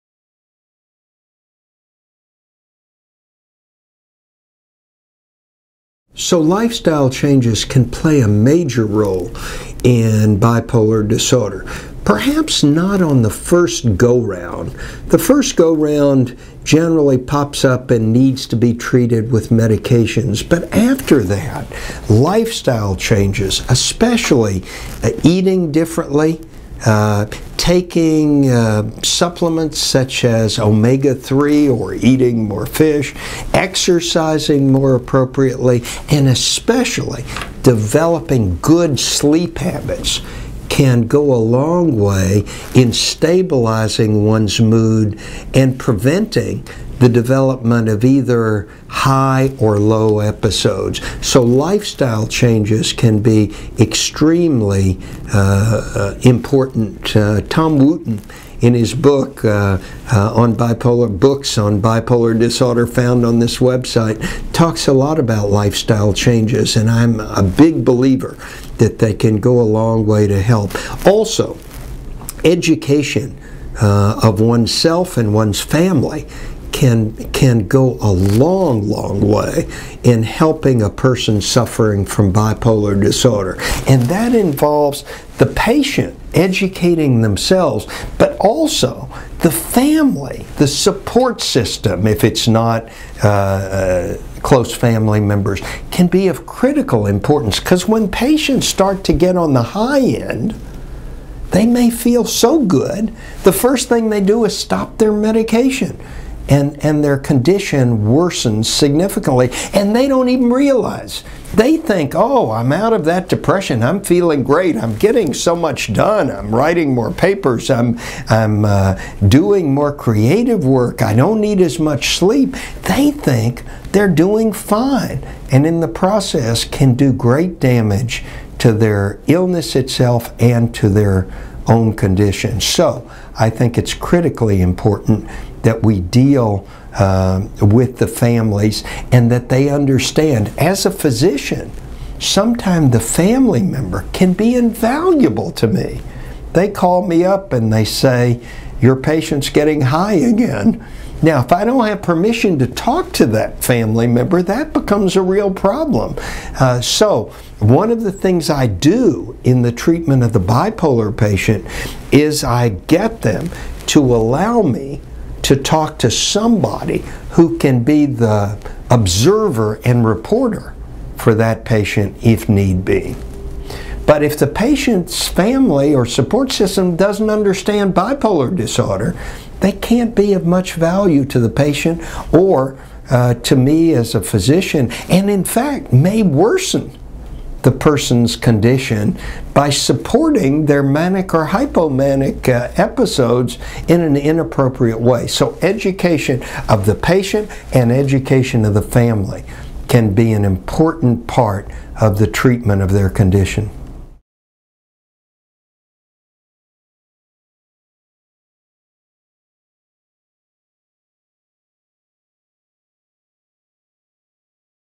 Bipolar Treatment: Lifestyle Changes Make Positive Difference
Lifestyle changes are an important part of a Bipolar Disorder treatment program. Learn which lifestyle changes can make a difference from HealthyPlace.com Medical Director, Dr. Harry Croft.

For comprehensive information on Bipolar Disorder, visit the HealthyPlace.com Bipolar Information Center